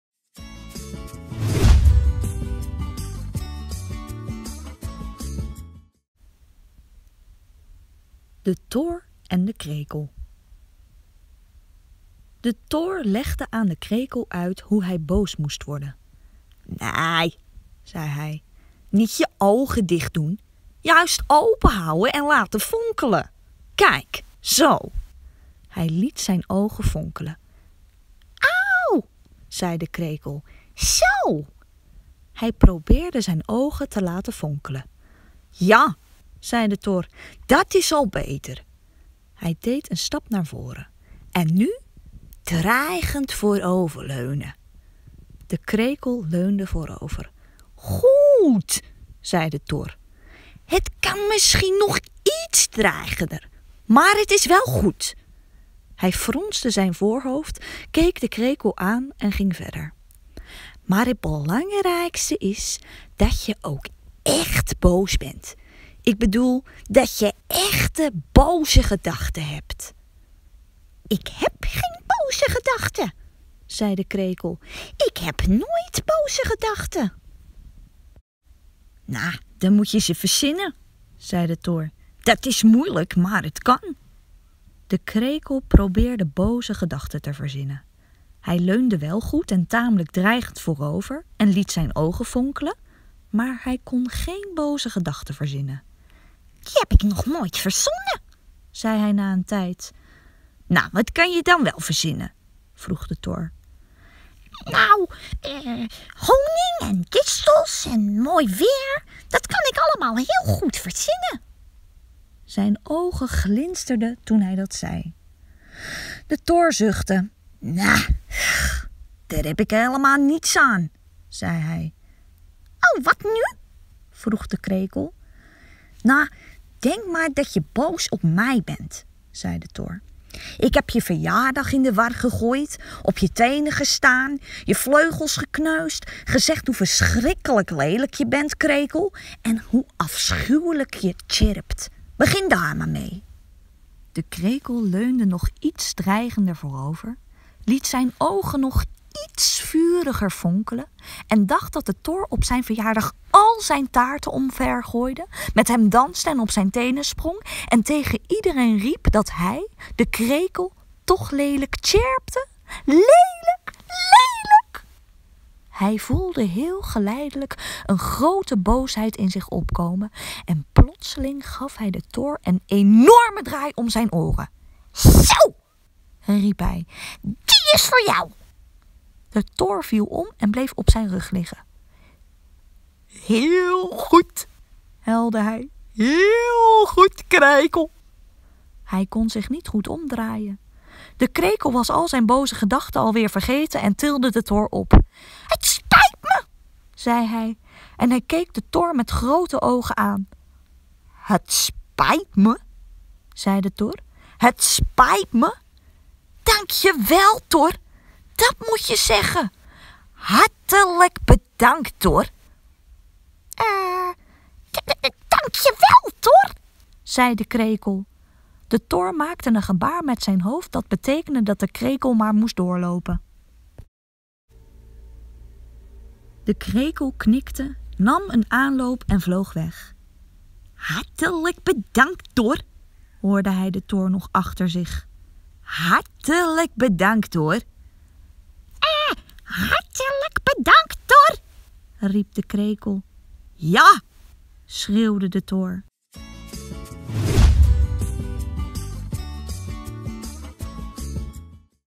DONSTHUIS: 'de tor en de krekel’ van Toon Tellegen
De tor leert de krekel boos te zijn.
Want hoe doe je dat nou eigenlijk?

Ingesproken door juf Marit